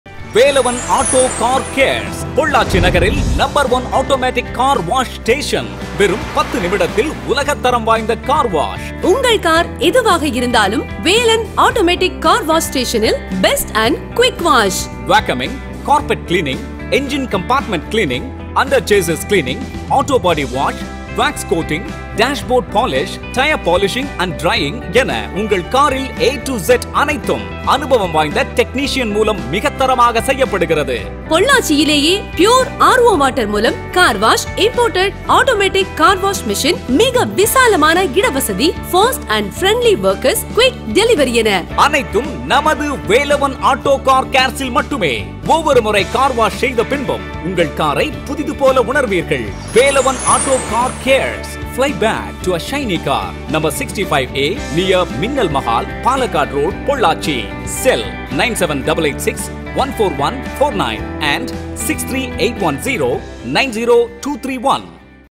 Velavan Aauto Car Care TVC | Pollachi
Velavan aauto Car wash, No:1 Automatic car wash station @ Pollachi surrounding! 10 mins Quick car wash and low price ever. Pure RO Water Carwash, Imported Automatic Car wash machine,  Fast & Friendly Workers, Quick Delivery! Fly back to a shiny car!!!